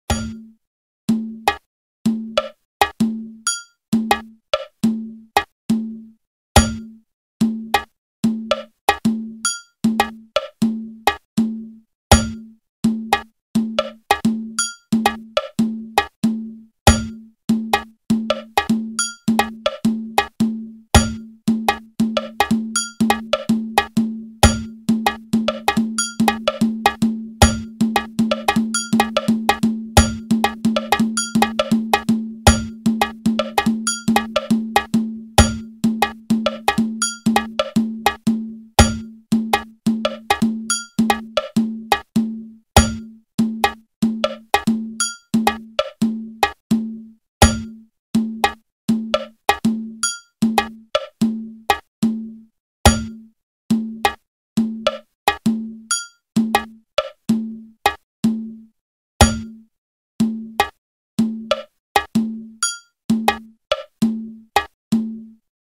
Changing Tempo Polyrhythm 7:5:3:2 with the Accessible Black on White high contrast skin
Shows the high contrast setting for Bounce Metronome. Look for Black on White in the Visuals drop menu.

This is one of the rhythms of the pro metronome.